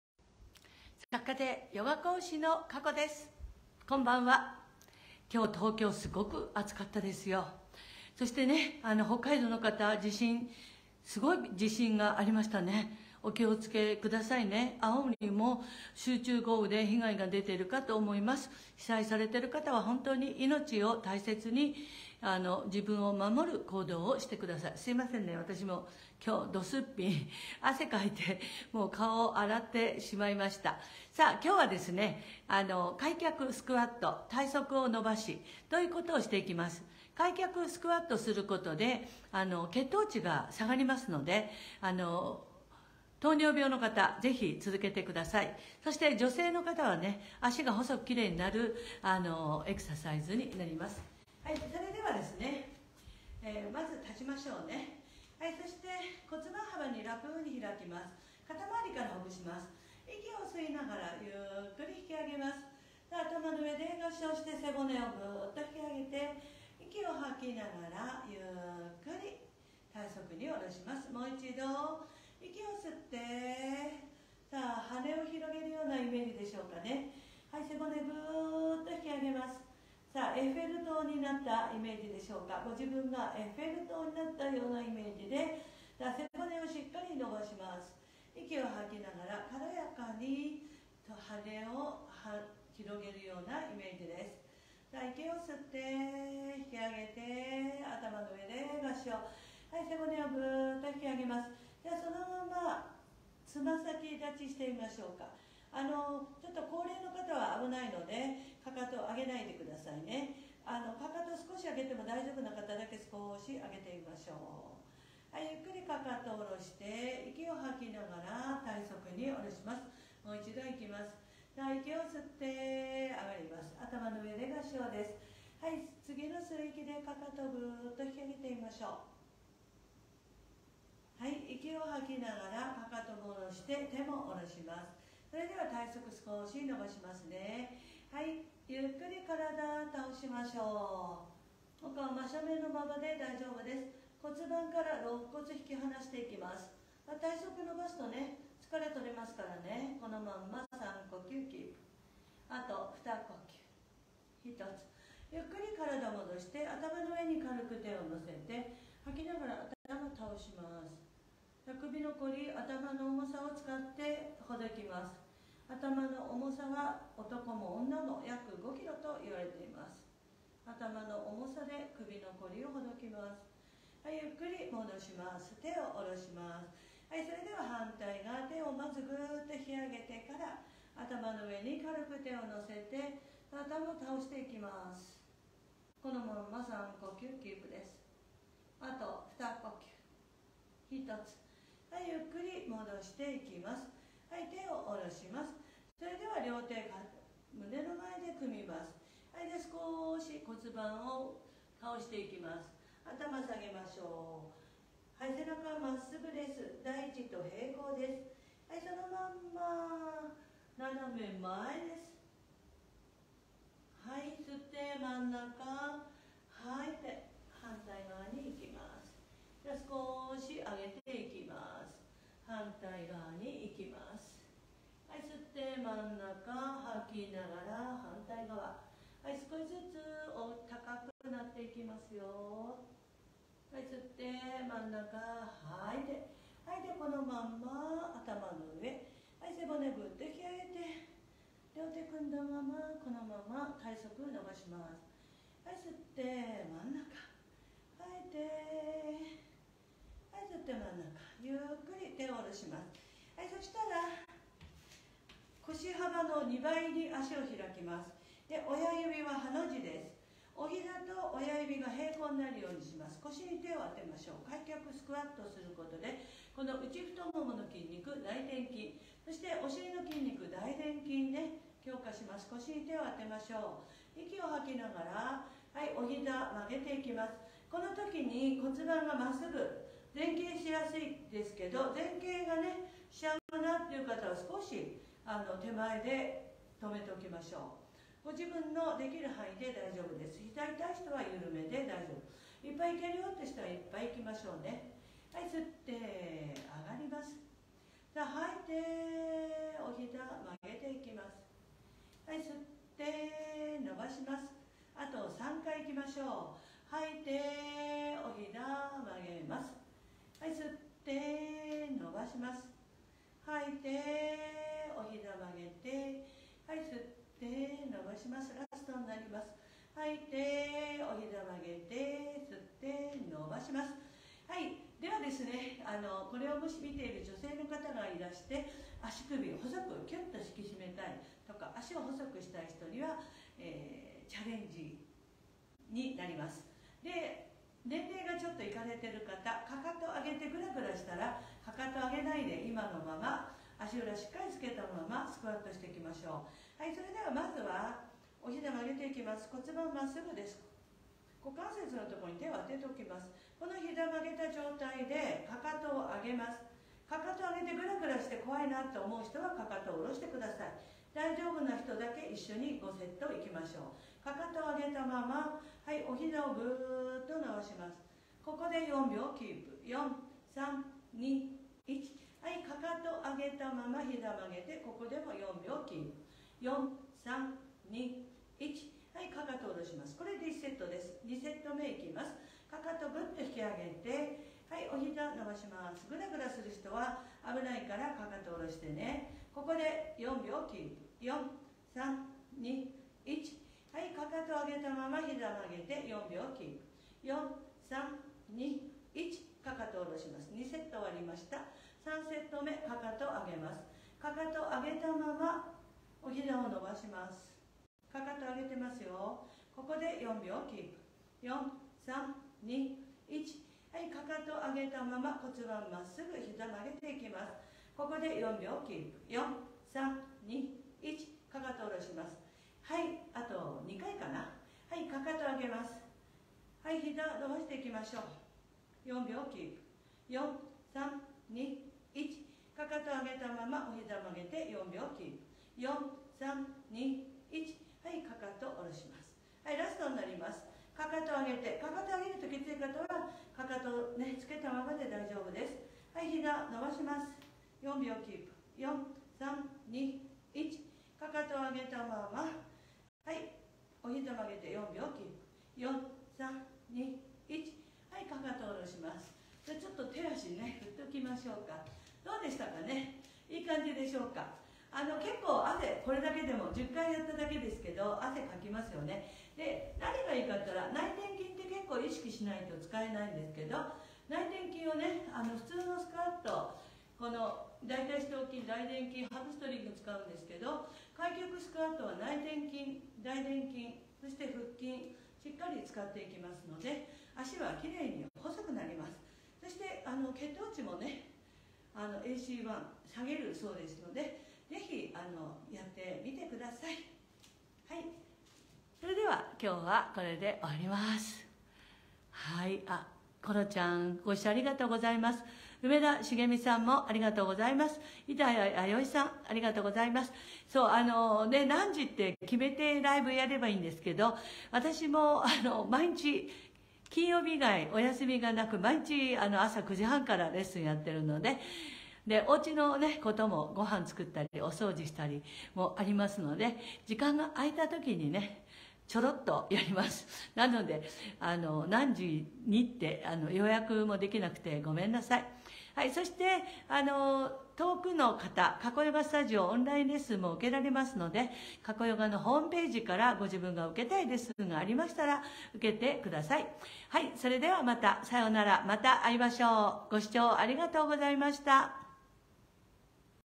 開脚スクワットで脚をきれいに！ 血糖値も下げる！
開脚スクワットで脚をきれいに！　血糖値も下げる！　KAKOYOGAスタジオ　南大沢

作家でヨガ講師kakoこと森本和子です。
開脚スクワットで、糖尿病の方、
血糖値を下げましょう。
女性のかたは、脚が細く綺麗になります。